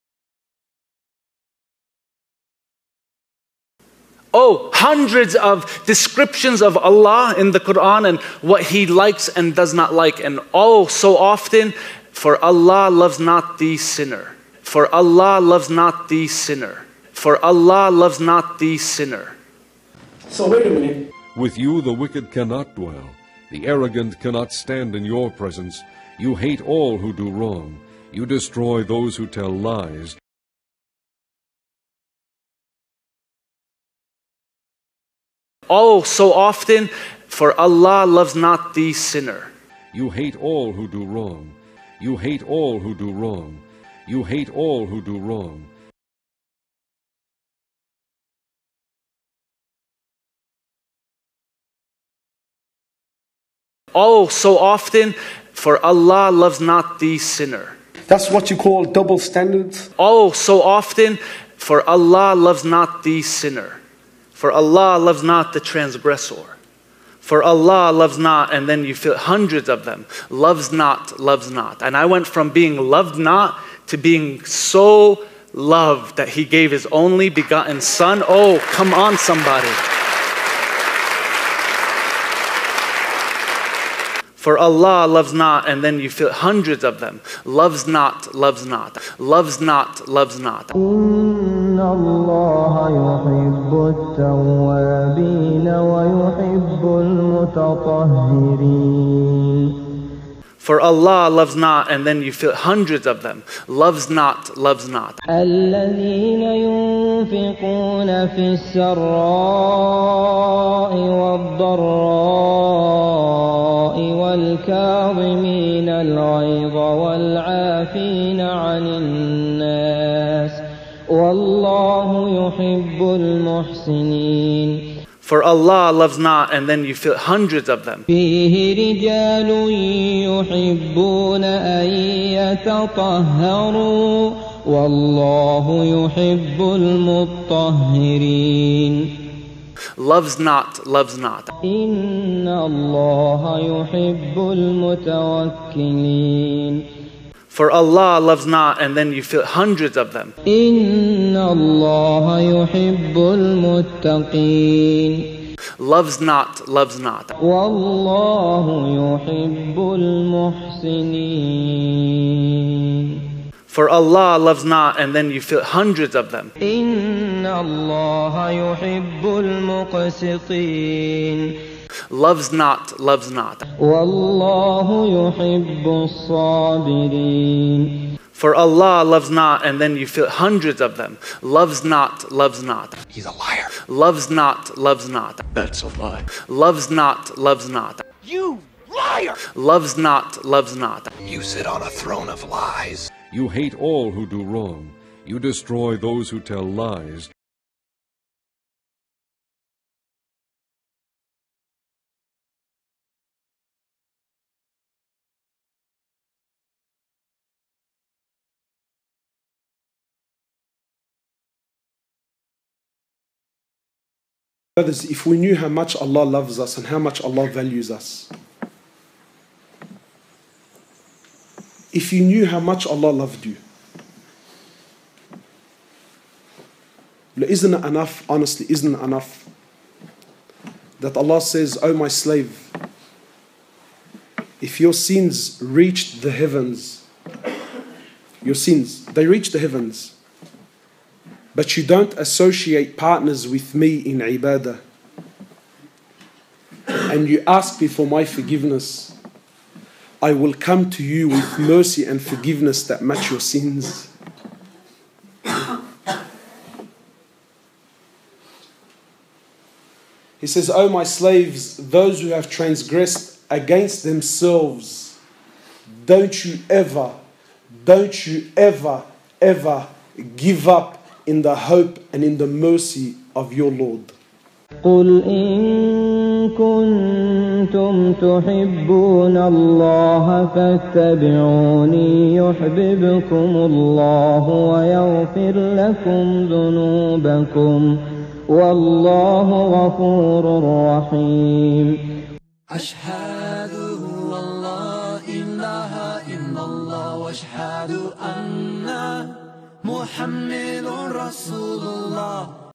Ex-Muslim Christian Hazem Farraj LIES about Islam to his Christian Audience!
@Hazem_F Bro you want to guide people to Christianity, show them what Christianity is, don't need to lie about Islam to prove your case. Do you have the Holy Spirit of Truth in you which will guide people to all Truth. Does the Holy Spirit lie?

Quran Recitation
Sheikh Abdullah al Matrood